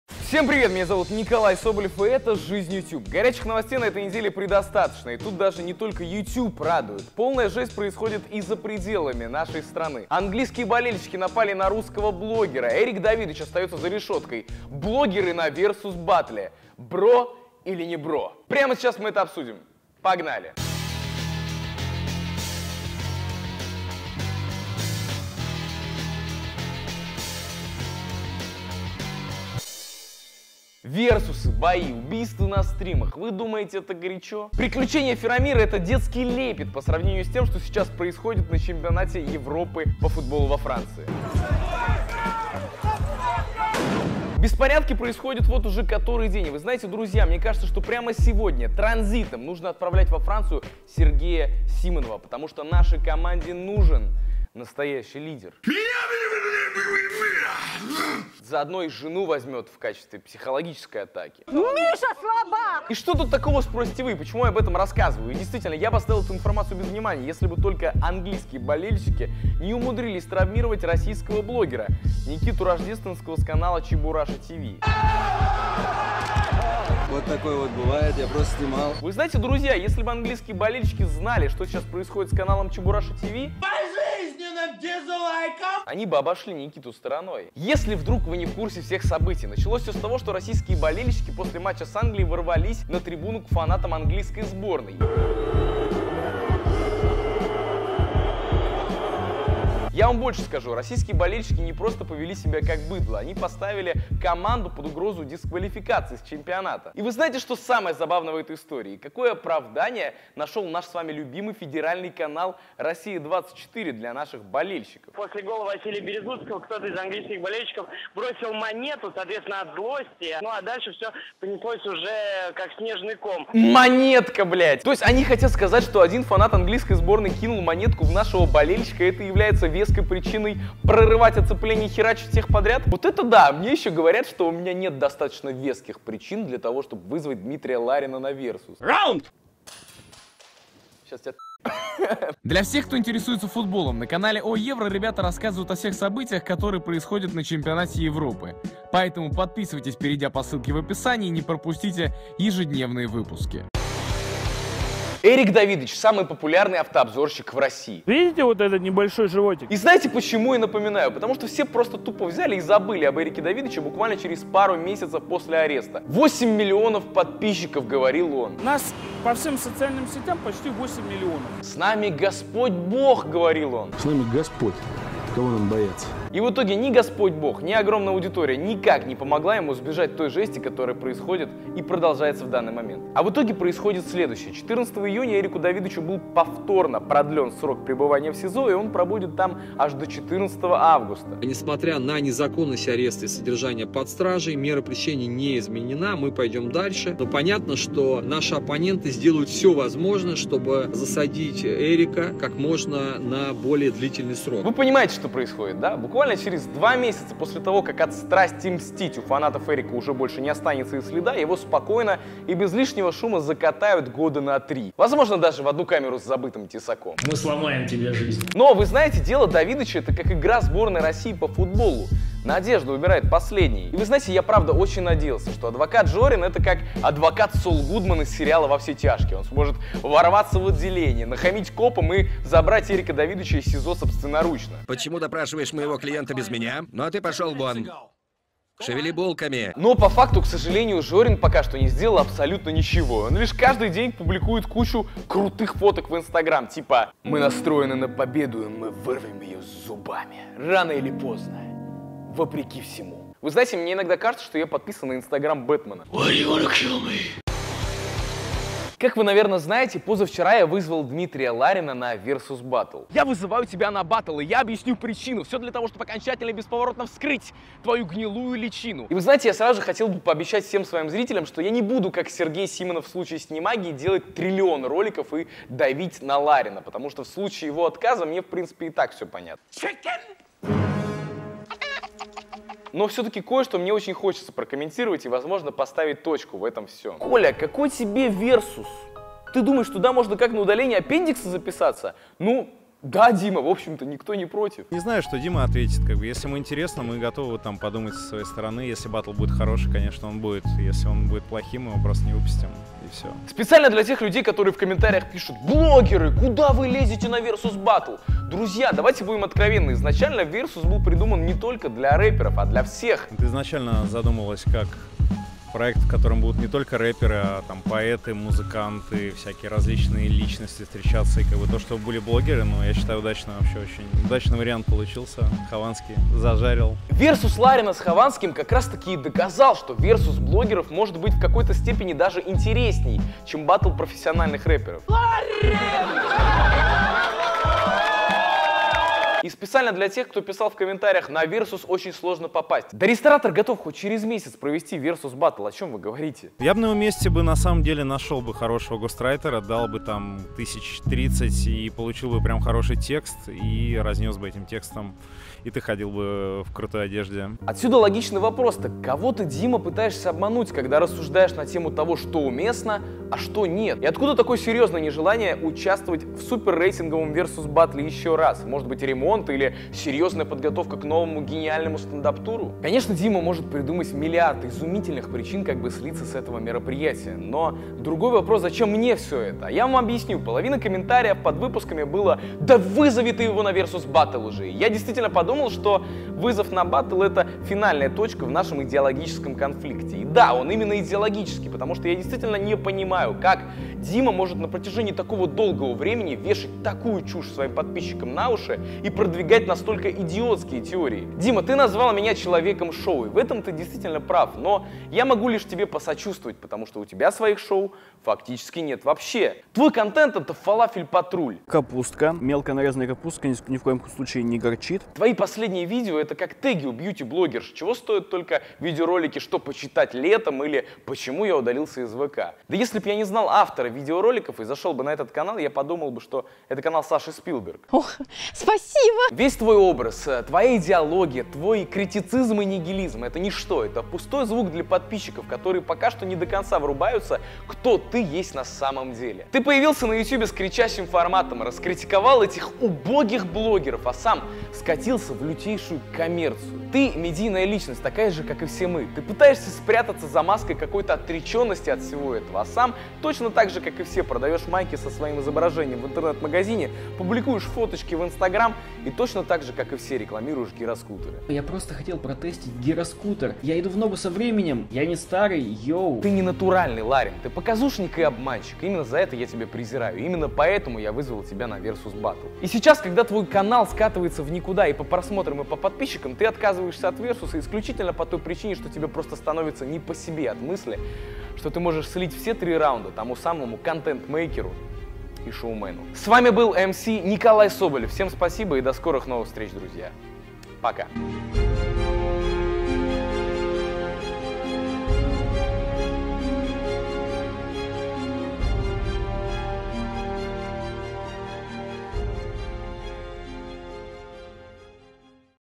ЖЮ#27 / Давидыч за решеткой, VERSUS: быть или не быть, Футбольная драка
Эрик Давидыч теряет надежду, блогеры на VERSUS: бро или не бро? Футбольные драки и травмы блогеров. Обсудим с Николаем Соболевым!

Спасибо за обрудование: kinoarenda.ru